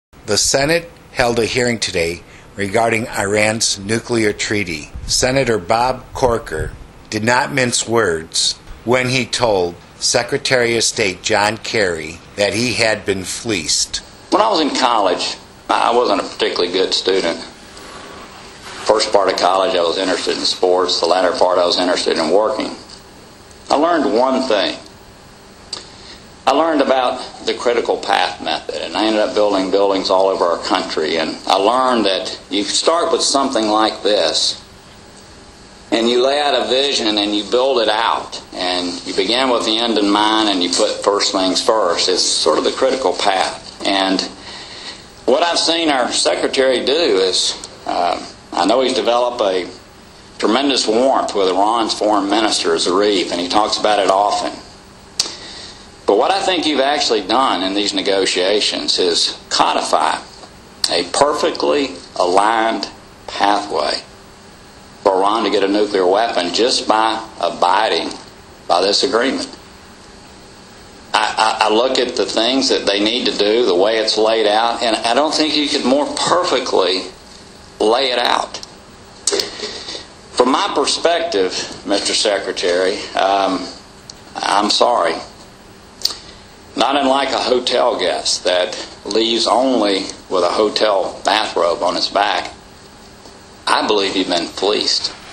Bob Corker tells John Kerry you have been fleeced
At today's hearing regarding Iran's ability to acquire a nuclear weapon listen to this commentary by Senator Bob Corker as he tells John Kerry you have been fleeced